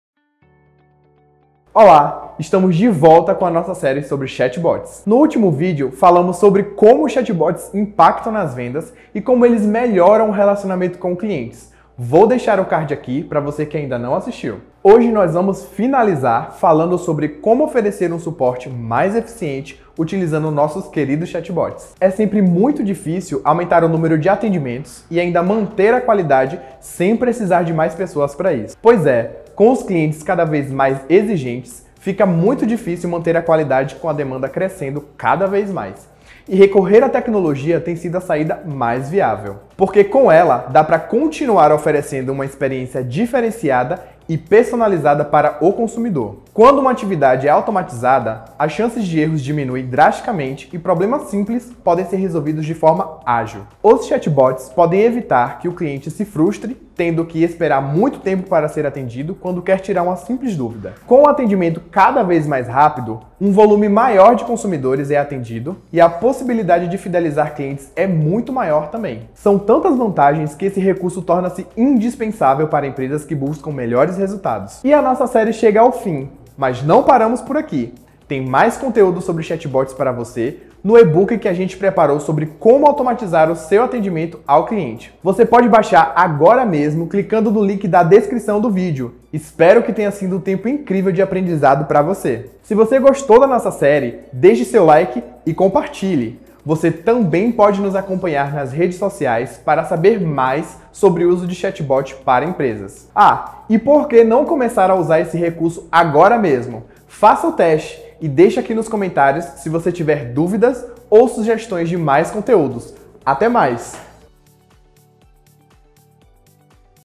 Série Chatbots: Suporte mais eficiente com chatbots (Episódio 5)
Saiba como oferecer um suporte mais eficiente utilizando o chatbot!

Esse é o último episódio da série sobre chatbots. Caso você não tenha visto os episódios anteriores, confira os links e aprenda mais sobre esse recurso!